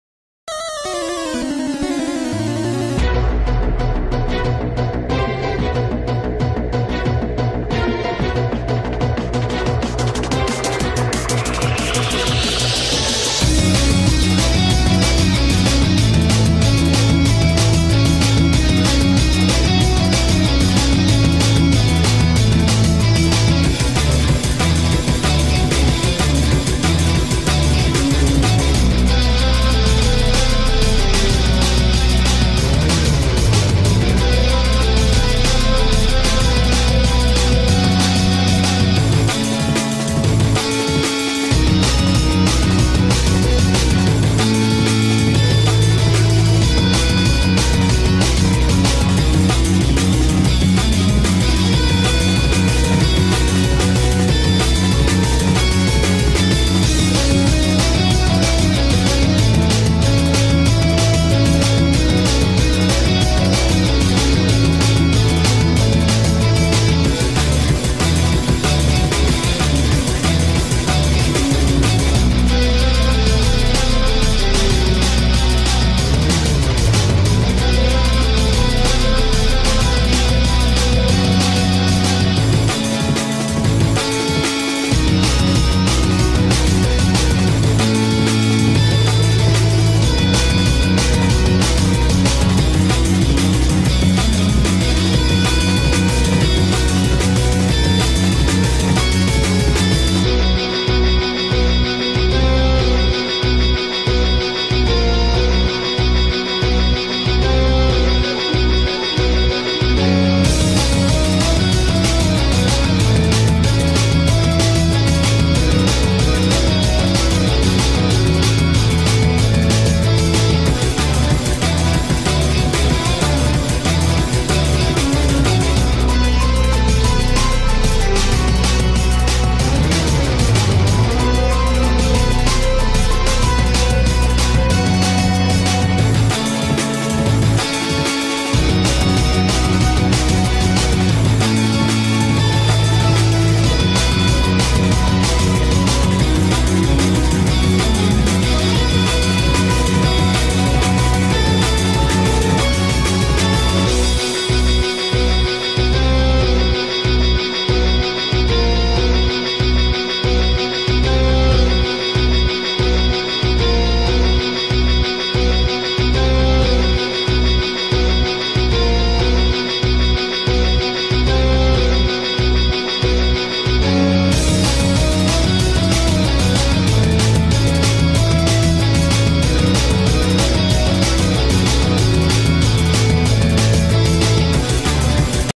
Top 10 Strongest Fire Type Pokemon
Fire Type is one of the strongest type of Pokemon universe with some strong Pokemon like Charizard, Blaziken, Reshiram ...